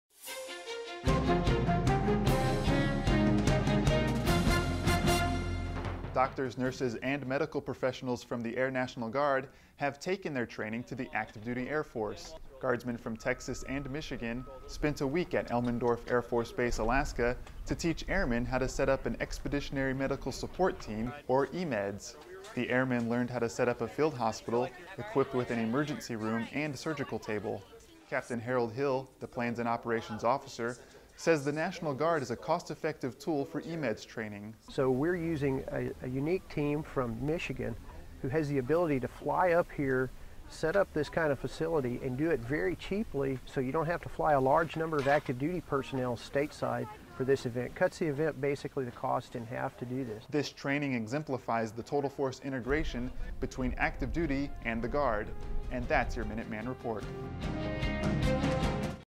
Air National Guard Trains Air Force in EMEDS
Airmen from the Michigan and Texas Air National Guard spent a week in Elemendorf Air Force Base, Alaska, to train Air Force Airmen how to set up an Expeditionary Medical Support team, or EMEDS. Story by Staff Sgt. Michael Crane